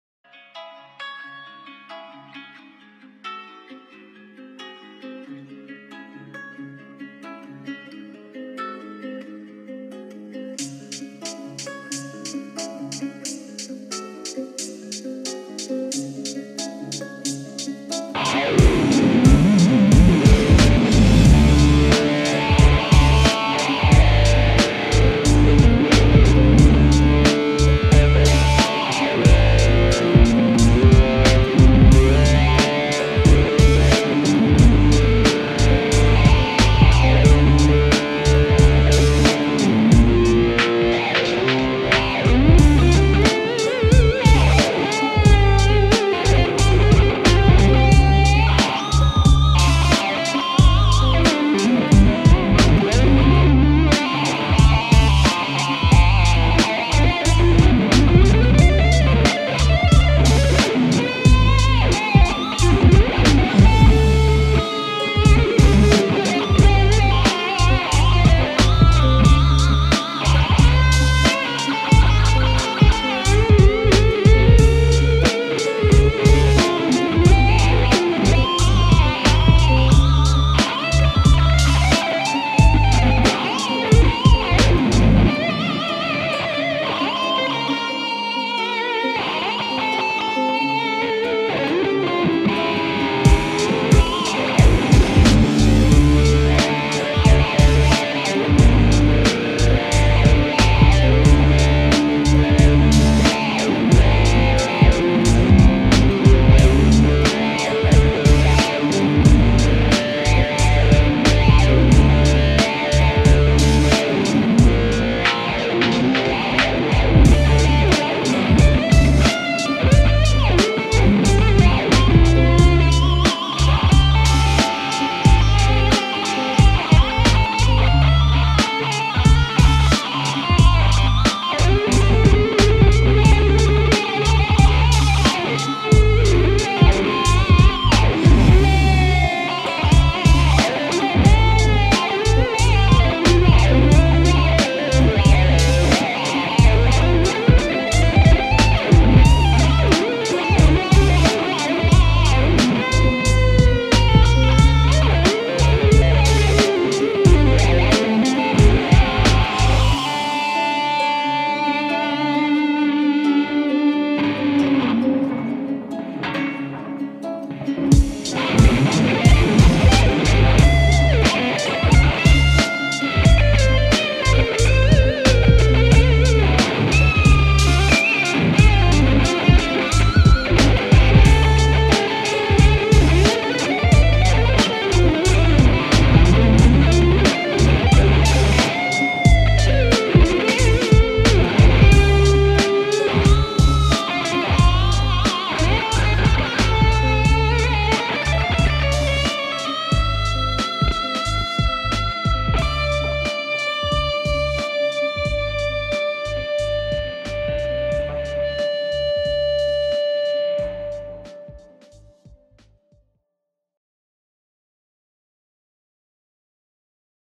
Jackson Sl2 USA meets the Ibanez Weeping Demon wah for a guitar shred fest.
Playing a Beautiful @jacksonguitarsusa SL2 Customshop guitarra "Mocha" ☕.
Mocha got a 3 peice Mahogany neck with Carbon fiber reinforcement Rods. Mahogany body with a black stained Maple top.
@seymourduncan  JB and 59.
German original @officialfloydrose5849
Ebony Board with Mother of Pearl sharktooth inlays.
Switch Craft Plug and selector switch.
Seymour Duncan Pots.
Using an Ibanez Weeping Demon 😈 as a wah.
Tones coming from the @McRocklin suit @polychromedsp VST.